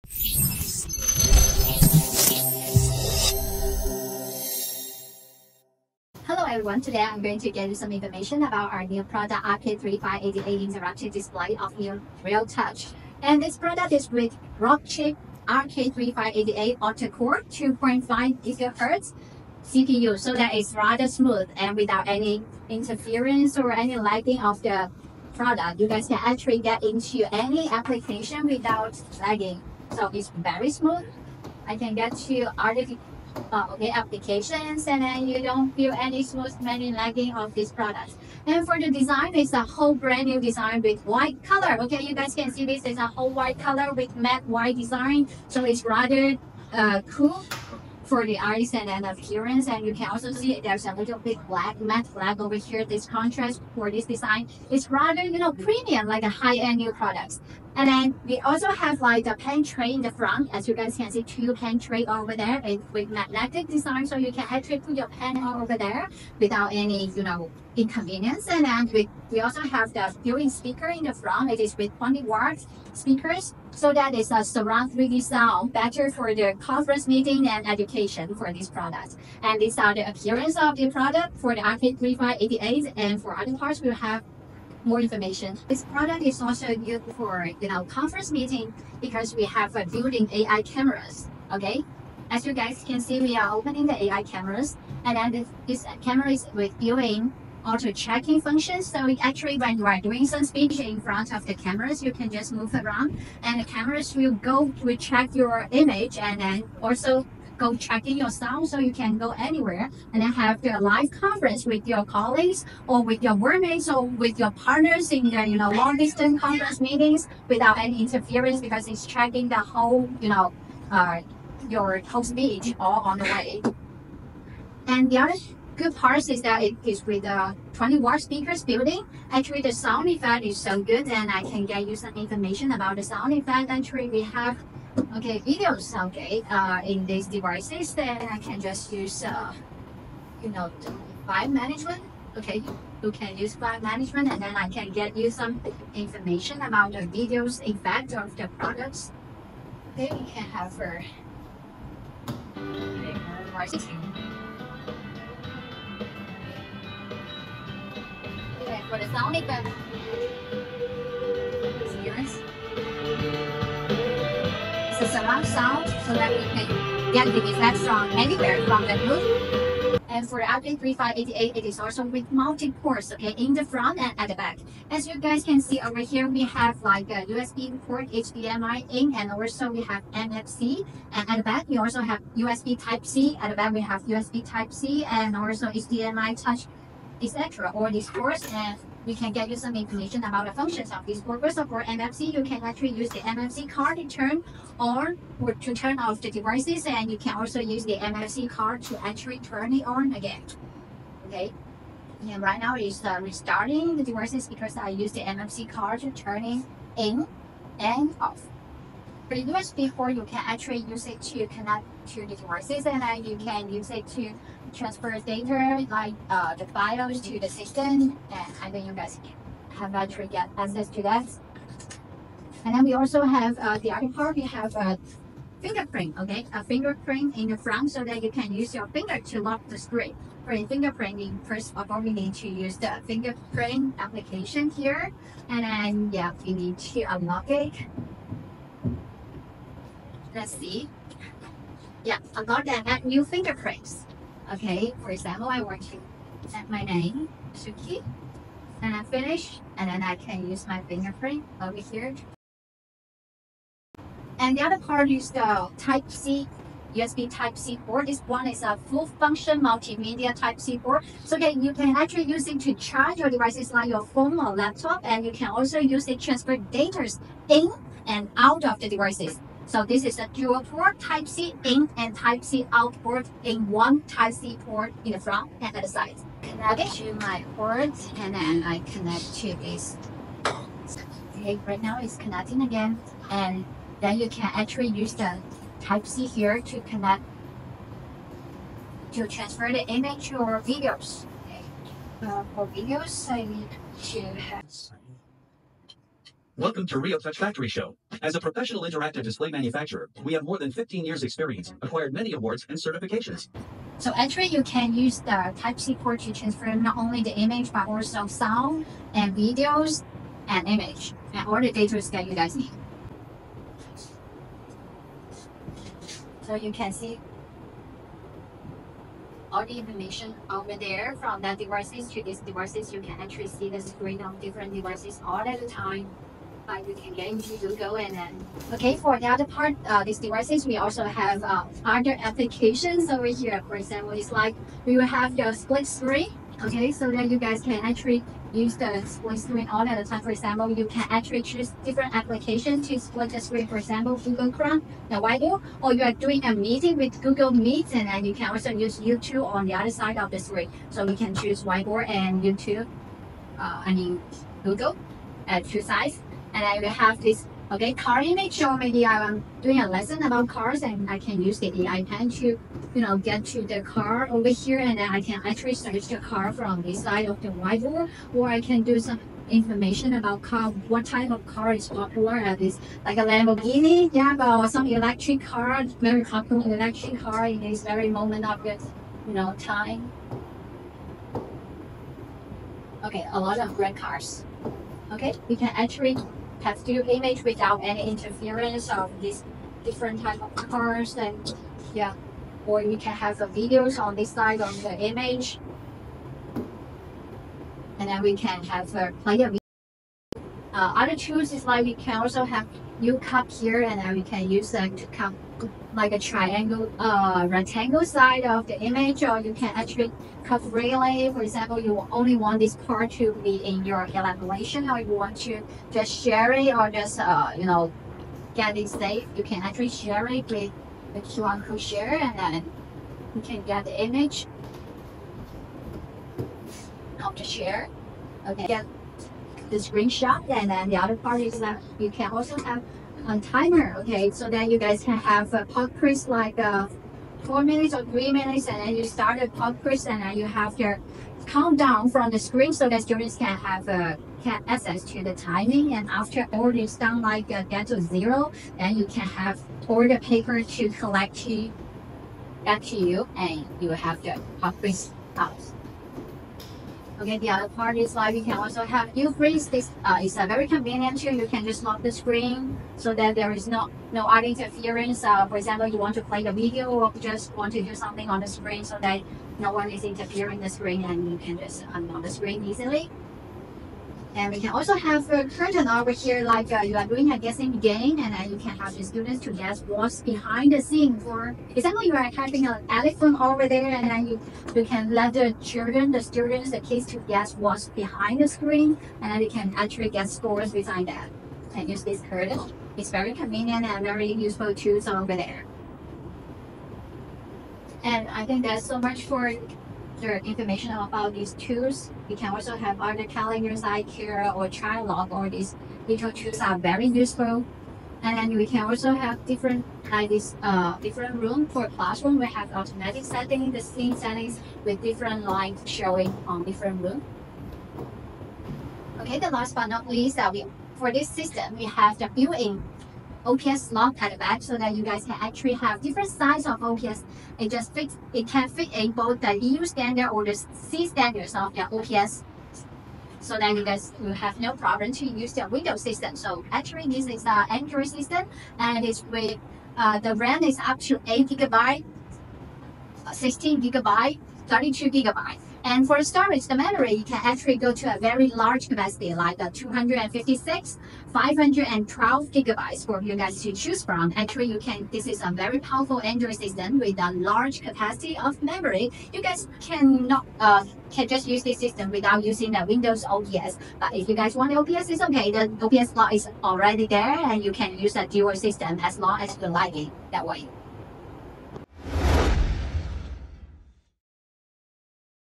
RK3588
Revolutionize the way you collaborate with the RioTouch RK3588 Interactive Display! Experience the power of an intuitive app-based user interface that makes teamwork a breeze. Whether in the classroom or the boardroom, our robust whiteboard functionality allows for real-time sharing and brainstorming, taking your productivity to new heights.